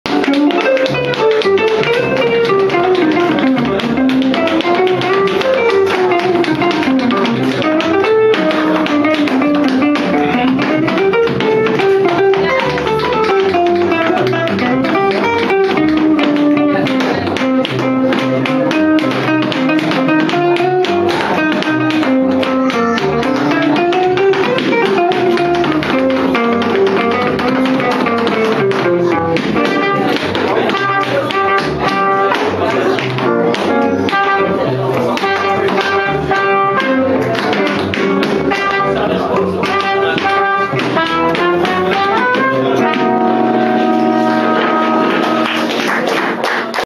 TapAquatre Malguenac 2015YT
La veille de l'ouverture du festival de Malguenac 2015, "le relax" avait avait invité un quartet original composé d'une danseuse de claquette (Patricia Blot) Luc Delhommeau (g) Alain Thézelais (cb) Adrien Faure (tp) autour d'un répertoire de standard de jazz.